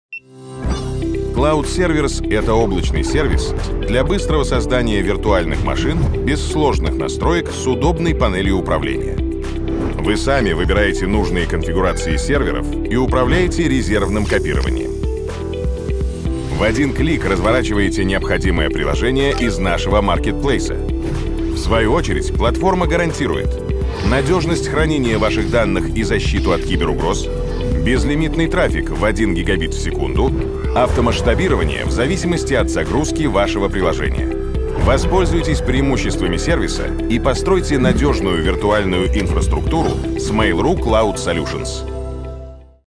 Mail.ru Cloud Servers — облачный сервис для создания виртуальных машин
Регистрируйтесь и тестируйте виртуальные серверы бесплатно Cloud Servers — это облачный сервис для создания виртуальных машин от Mail.ru Cloud Solutions, который позволяет быстро получить доступ к вычислительным ресурсам без сложных настроек и запустить виртуальную инфраструктуру для бизнеса.

Преимущества сервиса:
— Безлимитный трафик 1 Гбит/с
— Статический внешний (белый) IP-адрес
— Балансировщик нагрузки
— Автоматическое создание бекапов и управление образами
— Создание приватных сетей (VPC), настройка маршрутизации и фильтрации трафика, VPN